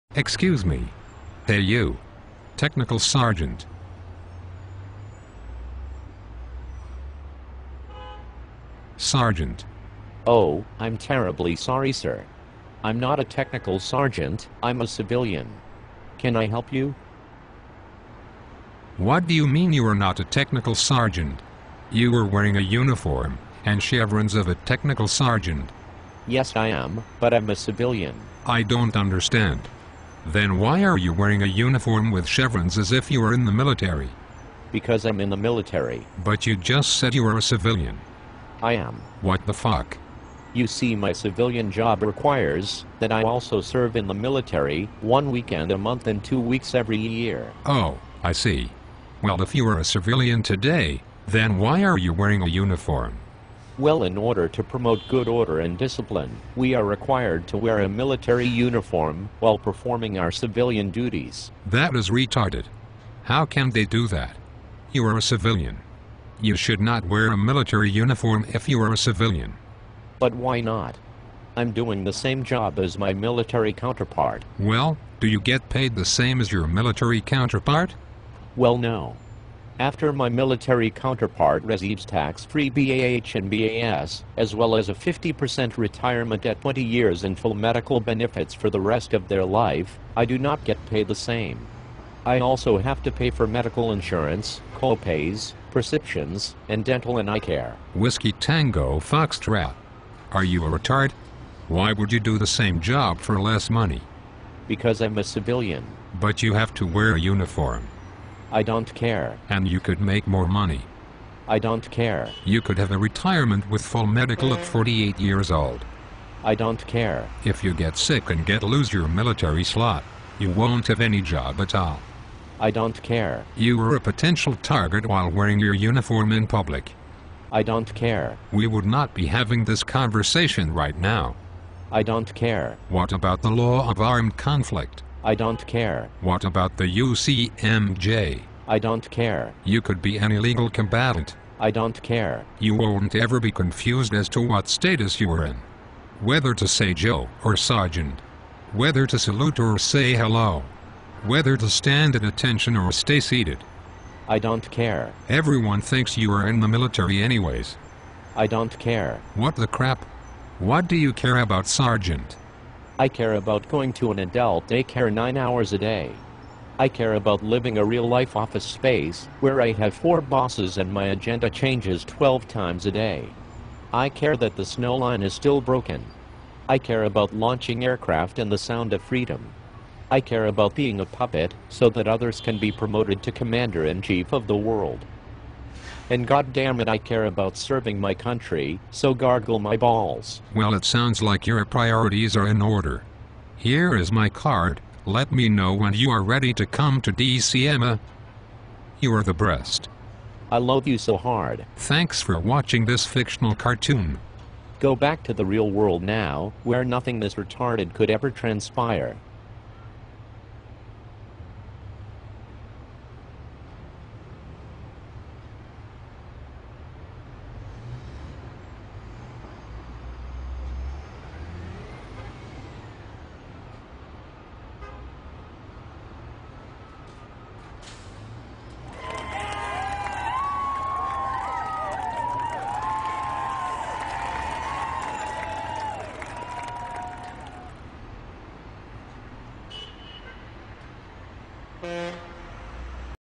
Uniforms for Life
The sad truth about civilian employees who are required to wear military uniforms.